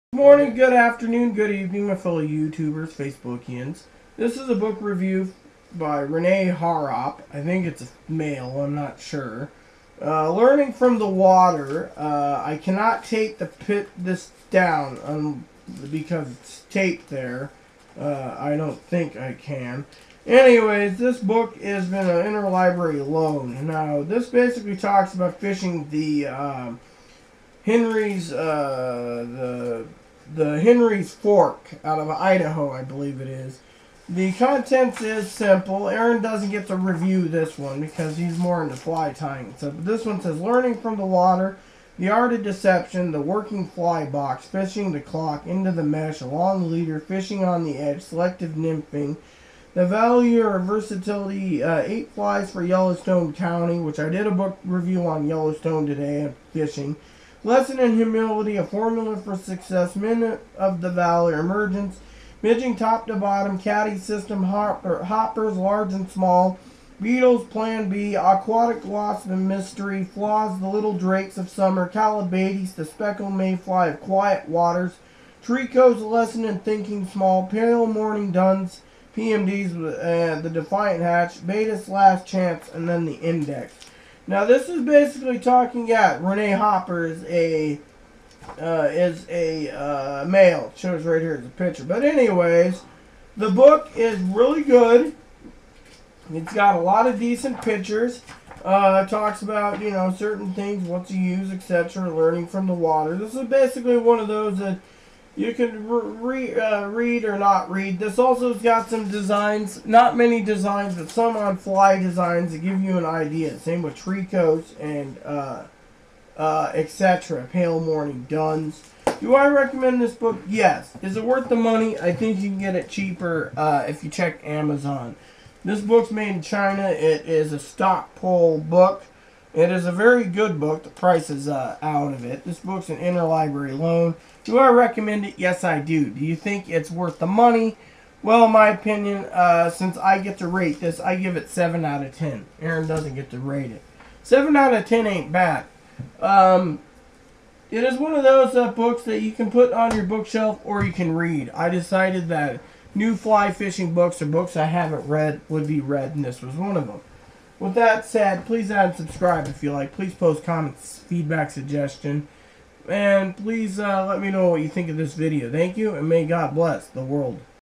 Learning from the water a Reene Harrop a book review
a book review Aeris gives it 7/10 a must read book